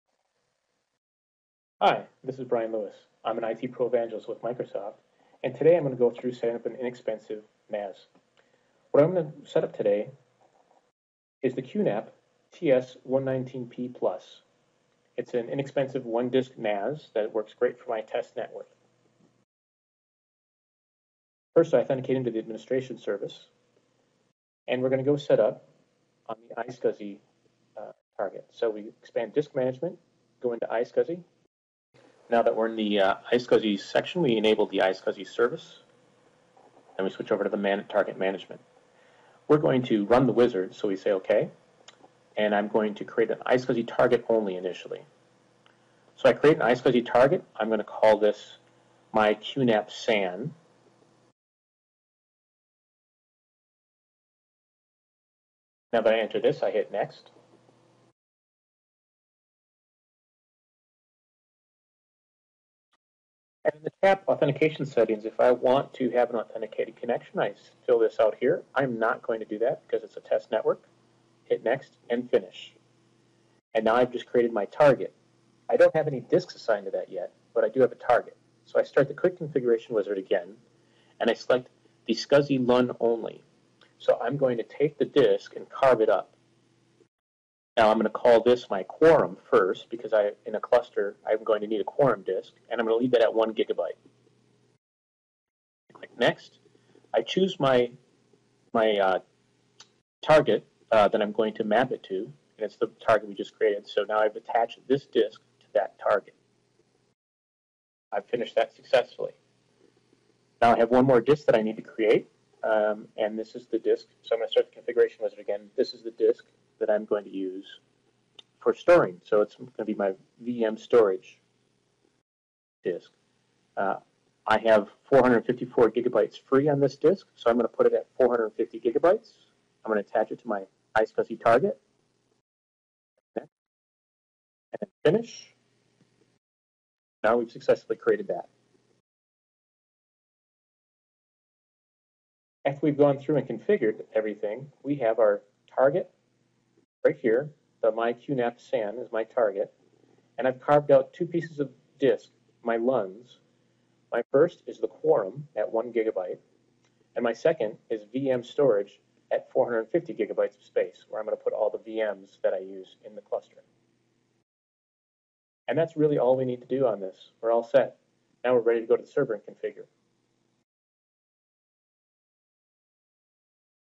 Configuring a QNAP iSCSI Target
A screencast of how to configure a QNAP iSCSI Target for use in a Hyper-v server farm.